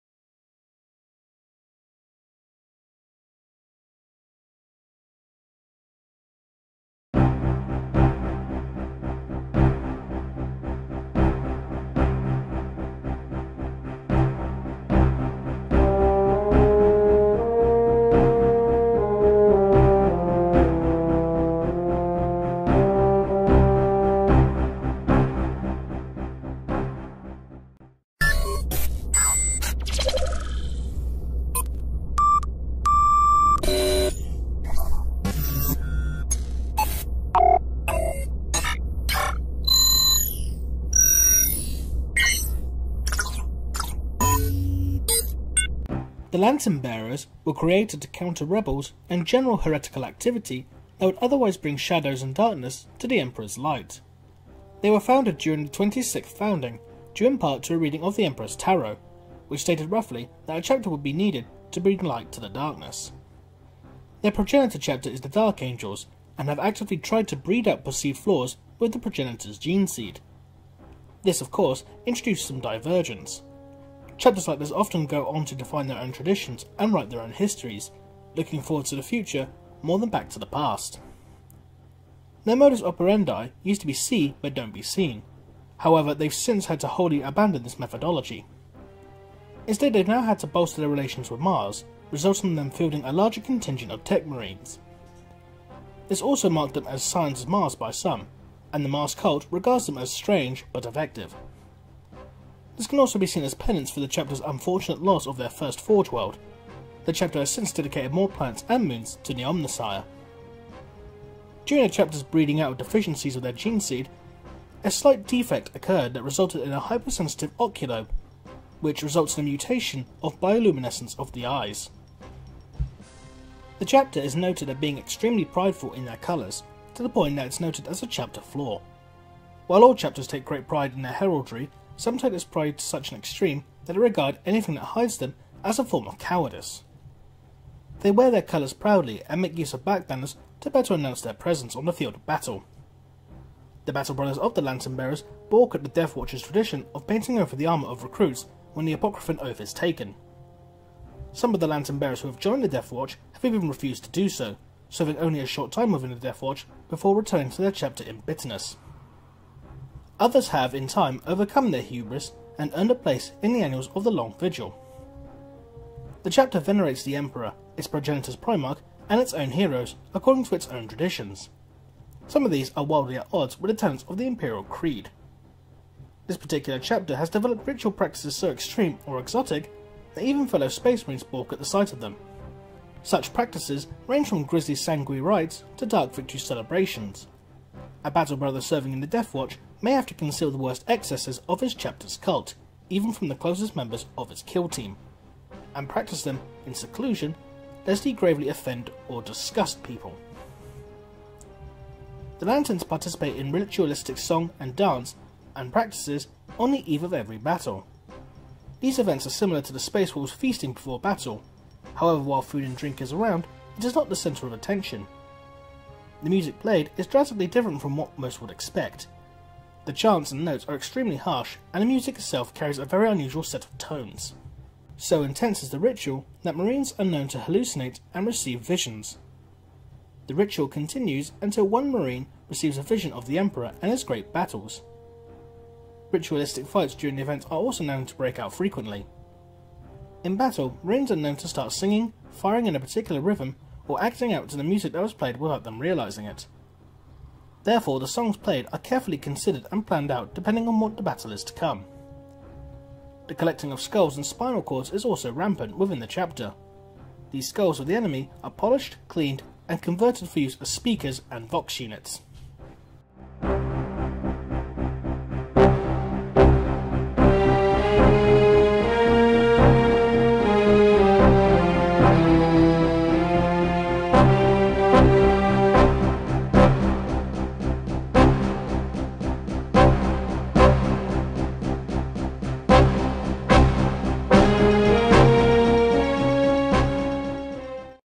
Homebrew Of The Week - Episode 20 - The Lantern Bearers
In this edition, we take a look at the Lantern Bearers submitted to us by Tyler Franck.
Artwork by David Sondered